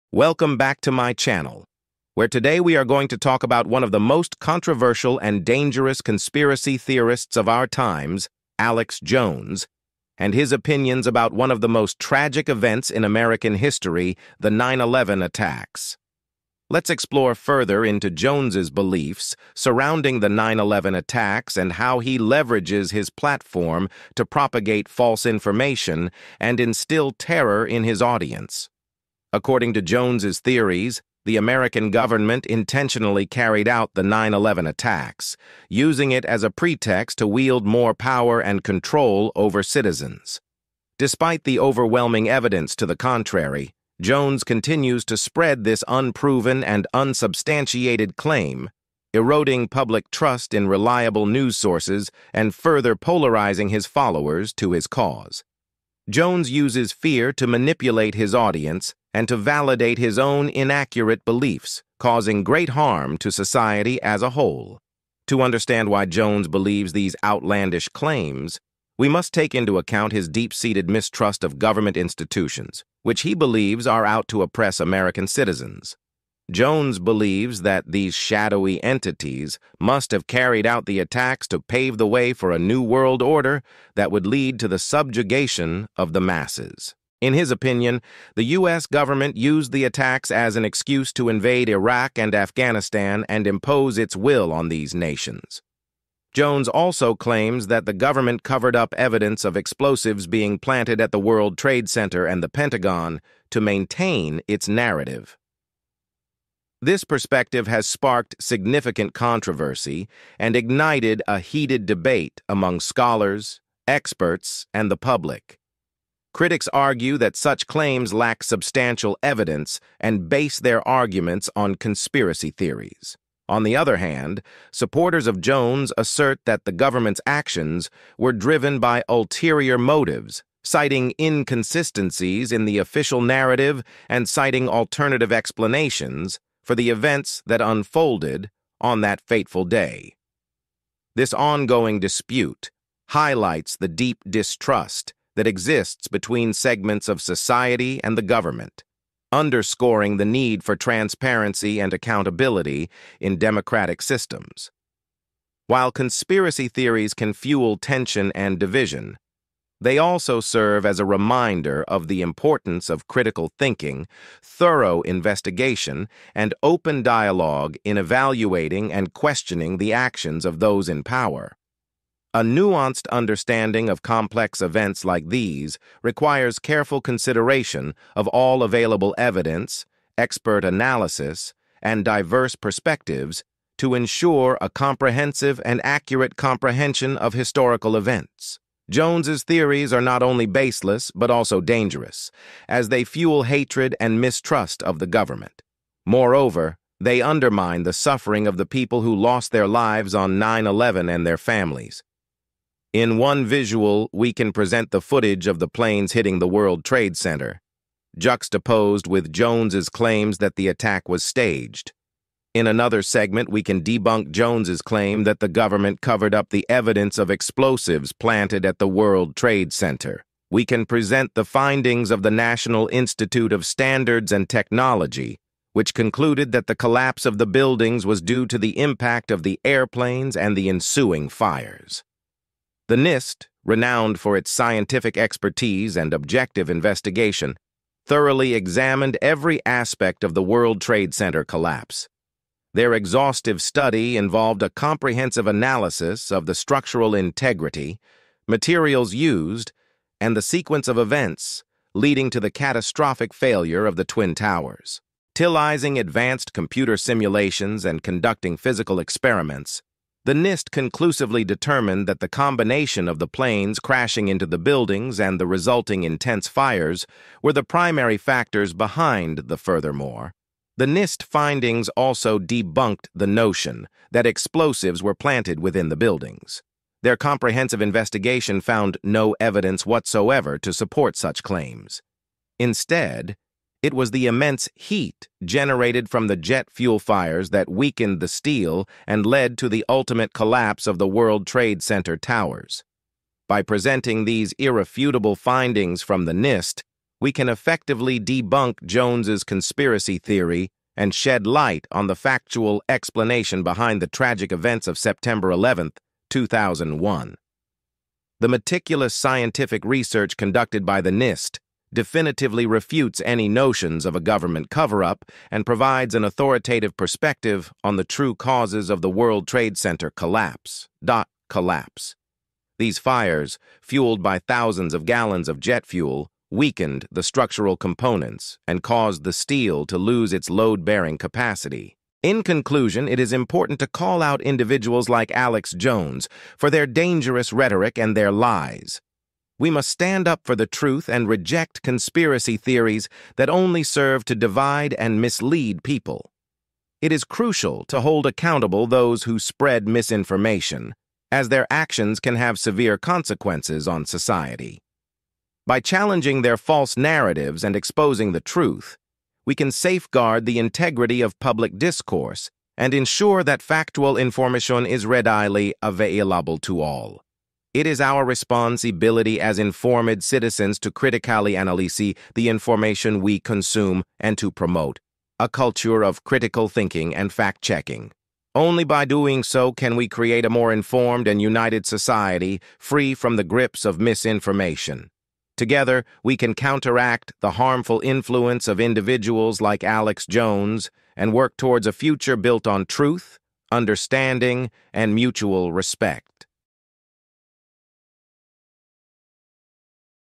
Cracking the Code: Alex Jones and the Mystery of 9/11!
This script video explores the enigmatic personality of Alex Jones and his theories on the events surrounding 9/11. Here, we investigate the backstory of one of the most controversial figures in contemporary American society and examine the evidence behind his claims.